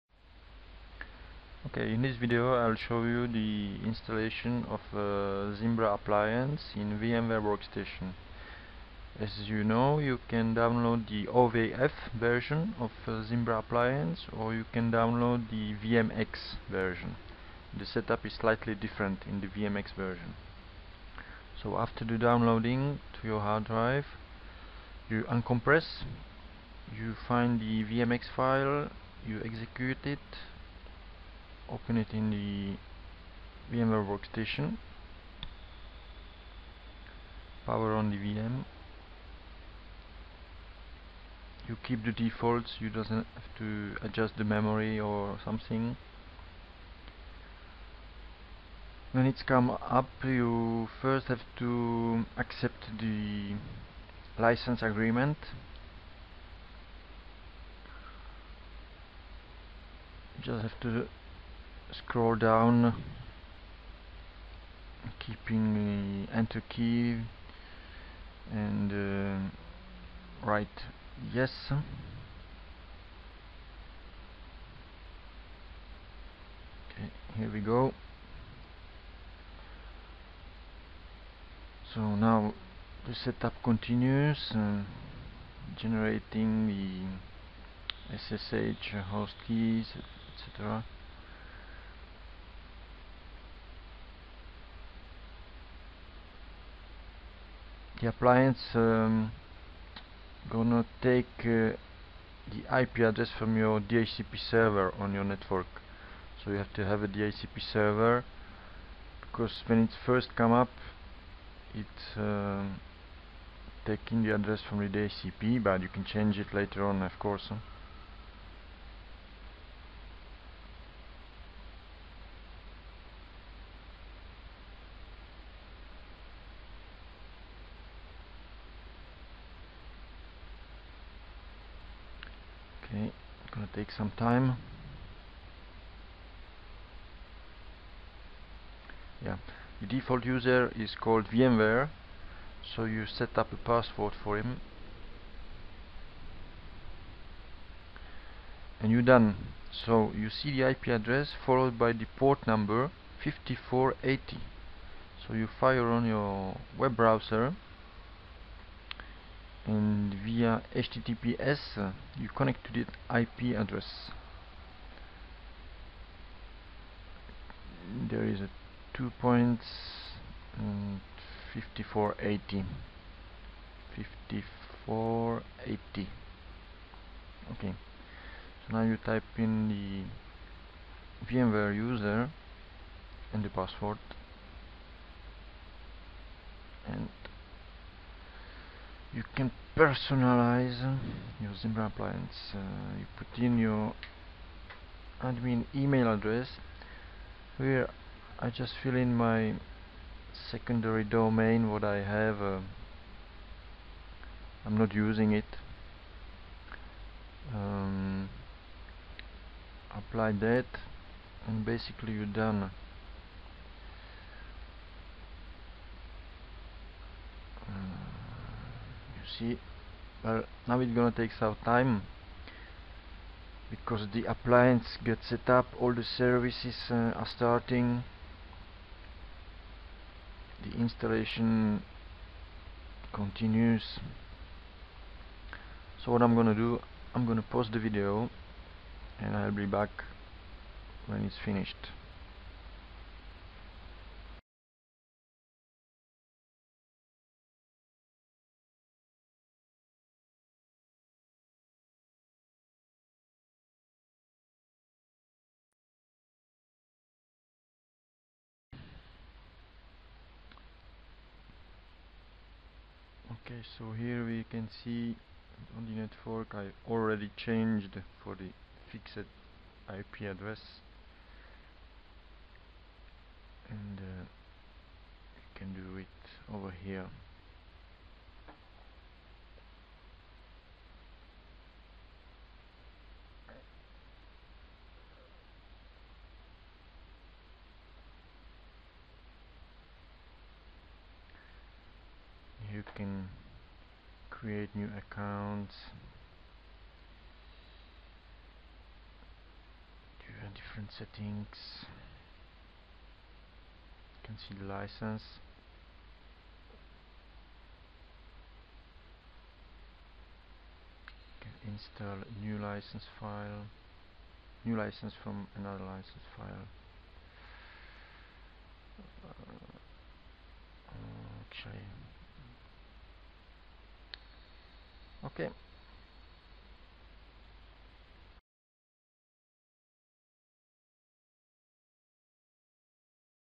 Installation Zimbra inside VMware Workstation 7.1
Installation and deployment of Zimbra Virtual appliance in VMware Workstation 7.1